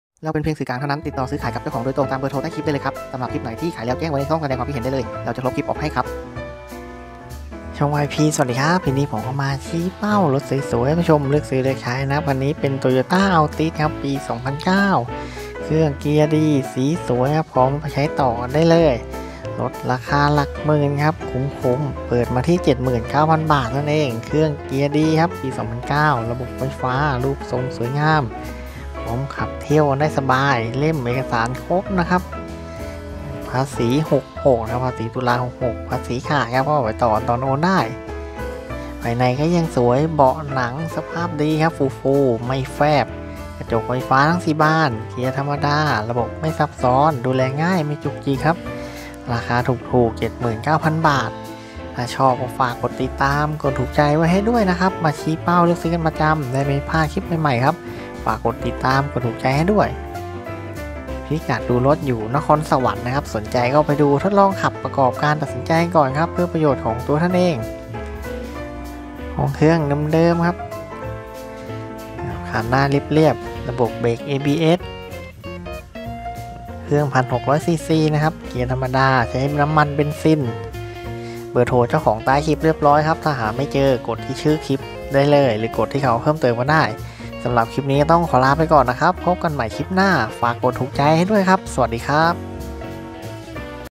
รถเก๋งราคาหลักหมื่น🎀🎉เครื่องเกียร์ d👍 สีสวย ขับทางไกลได้สบาย👑
2009 Toyota อัลติส
U79,000

 🚙💨Toyota altis เครื่อง1600cc ปี2009
เกียร์ธรรมดา   เชื้อเพลิงเบนซิน
👉Vายเงิuสด 79,000 เท่านั้น
👌ระบบไฟฟ้า
📍ไมค์  190952 โล
💥รถสวย,ขับทางไกลได้สบาย
⭐️เล่มพร้อมเอกสาร
💁‍♀️มาดูรถได้ที่สี่แยกไฟแดง พยุหะคีรี จ.นครสวรรค์
.ลองดู สินค้าอื่น